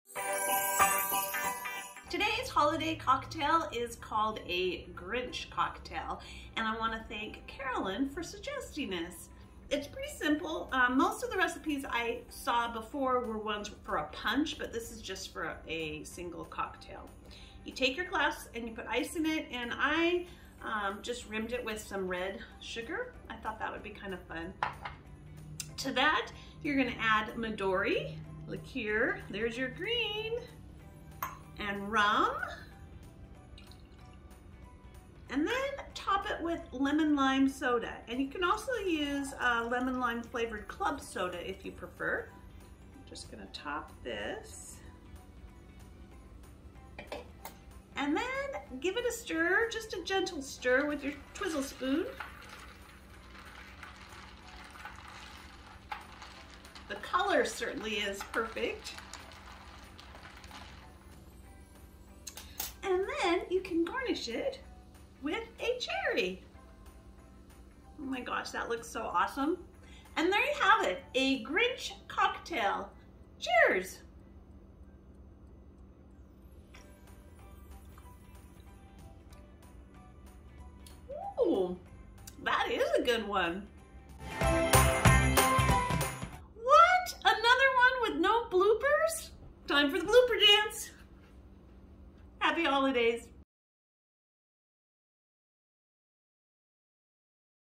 The Grinch Cocktail
A flavorful and eye-catching cocktail for the holidays!
Kitchen Products: pamperedchef.biz/lindairwin